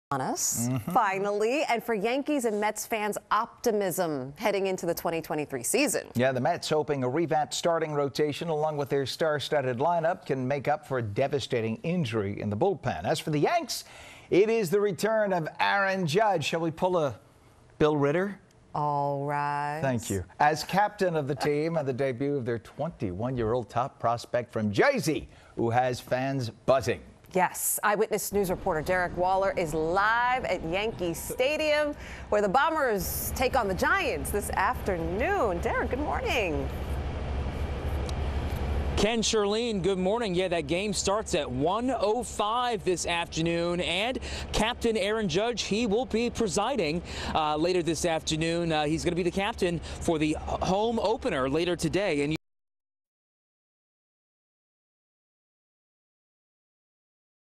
Aaron Judge returns for Yankees opening day
New York Yankees take on San Francisco Giants for home opener. Derick Waller reports from Yankee Stadium.